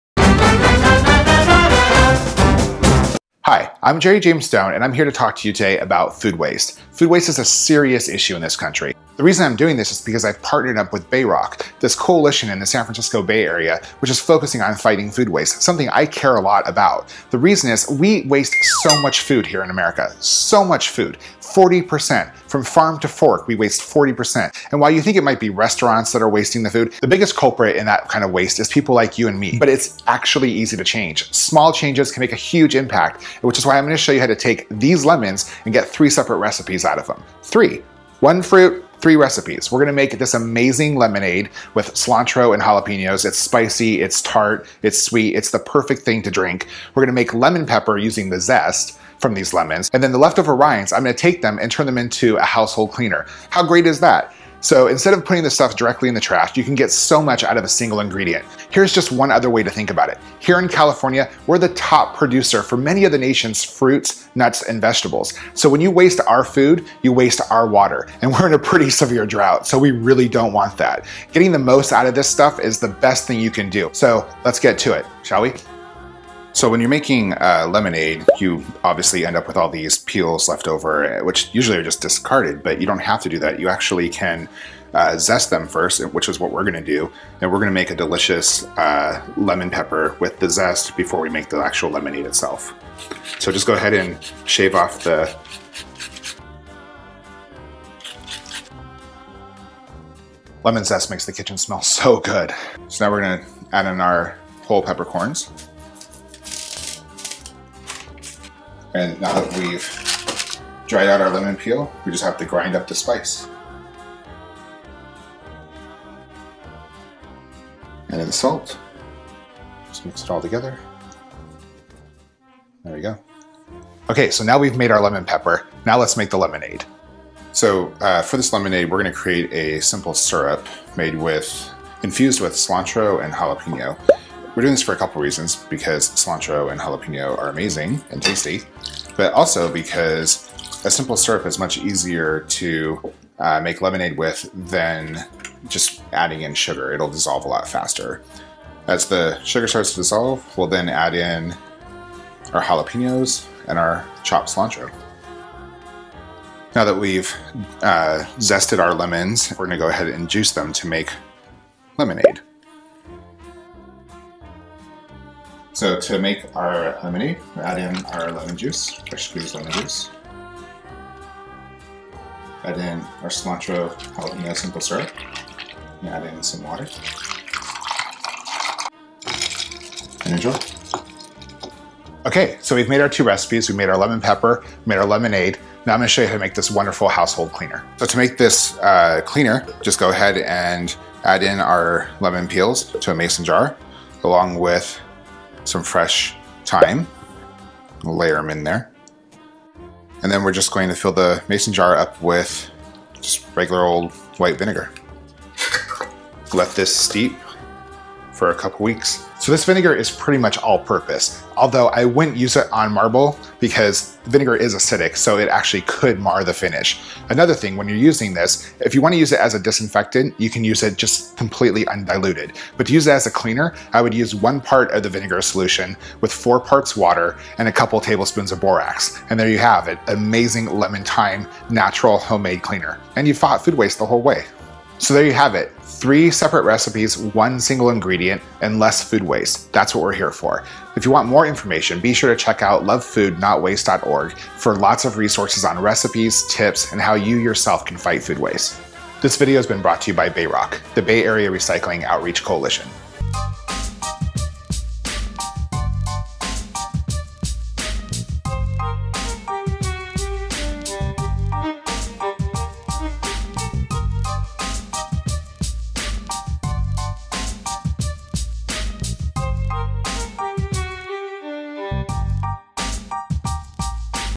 Lemon Three Ways: Spicy Lemonade, Lemon Pepper, DIY Lemon Thyme Cleaner
Easily fight food waste with Root to Leaf Cooking, using every part of an ingredient. In this video you'll learn how to turn a batch of lemons into lemon pepper, lemonade, and a homemade natural lemon cleaner. Three recipes with the same batch of lemons!